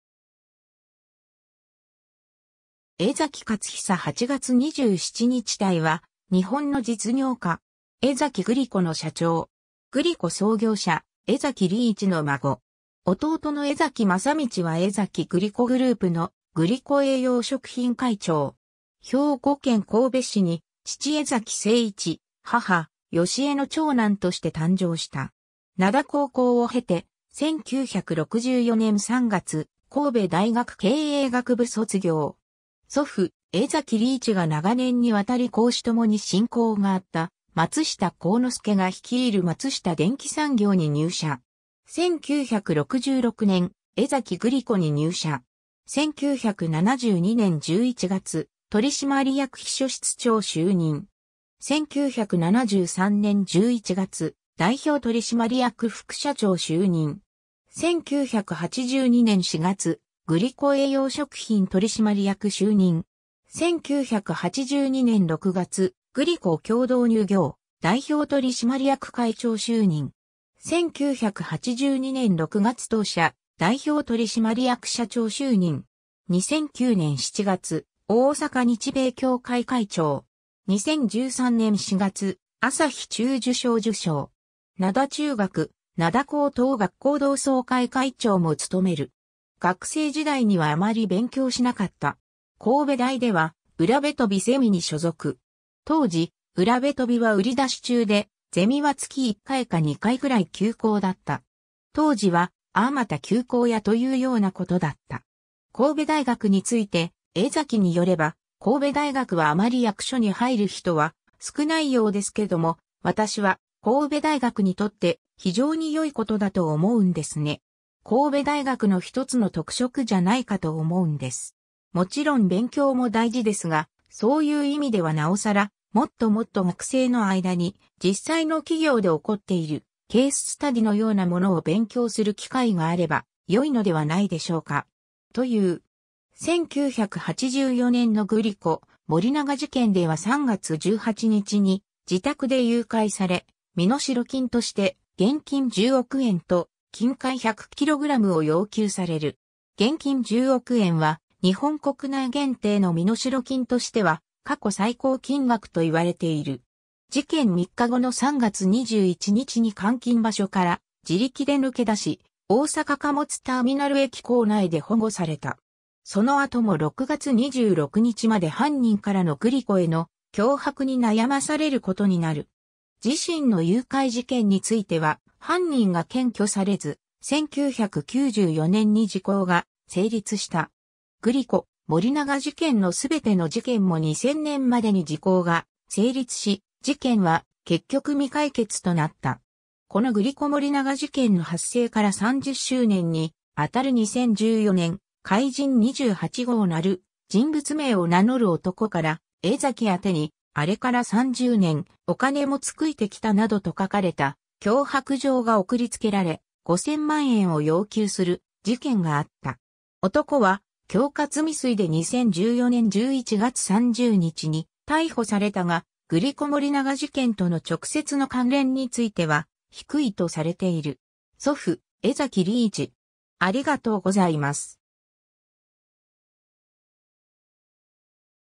江崎勝久
江崎 勝久（えざき かつひさ、1941年（昭和16年）8月27日 - ）は、日本の実業家。
江崎グリコの社長。
 グリコ創業者・江崎利一の孫。
弟の江崎正道は江崎グリコグループのグリコ栄養食品会長。
 兵庫県神戸市に父江崎誠一、母芳江の長男として誕生した。
灘高校（新制第12回）を経て、1964年3月、神戸大学経営学部卒業。
祖父江崎利一が長年にわたり公私ともに親交があった松下幸之助が率いる松下電器産業（現・パナソニック）に入社。
1966年、江崎グリコに入社。
 1972年11月、取締役秘書室長就任。
1973年11月、代表取締役副社長就任。
1982年4月、グリコ栄養食品㈱取締役就任。
1982年6月、グリコ協同乳業㈱（後のグリコ乳業㈱）代表取締役会長就任。
1982年6月当社代表取締役社長就任。
 2009年7月、大阪日米協会会長。
2013年4月、旭日中綬章受章。
 灘中学・灘高等学校同窓会会長も務める。
 学生時代にはあまり勉強しなかった。
神戸大では占部都美ゼミに所属。
当時、占部都美は売り出し中で、ゼミは月1回か2回ぐらい休講だった。
当時は「ああまた休講や」というようなことだった。
神戸大学について江崎によれば「神戸大学はあまり役所に入る人は少ないようですけども、私は神戸大学にとって非常に良いことだと思うんですね。神戸大学の１つの特色じゃないかと思うんです。もちろん勉強も大事ですが、そういう意味ではなおさら、もっともっと学生の間に実際の企業で起こっているケーススタディのようなものを勉強する機会があればよいのではないでしょうか。」という。
 1984年のグリコ・森永事件では3月18日に自宅で誘拐され、身代金として現金10億円と金塊100kgを要求される。
現金10億円は日本国内限定の身代金としては過去最高金額と言われている。
事件3日後の3月21日に監禁場所から自力で抜け出し、大阪貨物ターミナル駅構内で保護された。
その後も6月26日まで犯人からのグリコへの脅迫に悩まされることになる。
 自身の誘拐事件については犯人が検挙されず、1994年に時効が成立した。
グリコ・森永事件のすべての事件も2000年までに時効が成立し、事件は結局未解決となった。
 このグリコ森永事件の発生から30周年に当たる2014年、「怪人28号」なる人物名を名乗る男（年齢・氏名不詳）から、江崎宛に「あれから30年、お金も尽いてきた」などと書かれた脅迫状が送りつけられ、5000万円を要求する事件があった。
男は恐喝未遂で2014年11月30日に逮捕されたが、グリコ森永事件との直接の関連については低いとされている。
 祖父江崎利一